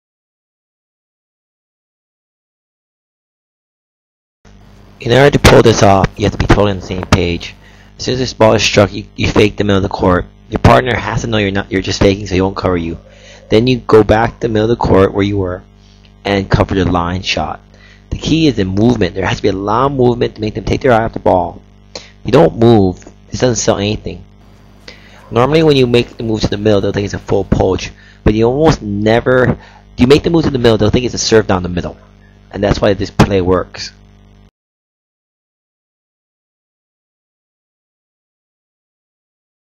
DANGEROUS Plays: Wide Serve Poach
wants to remind people that every shot requires the net person to move...poach or no poach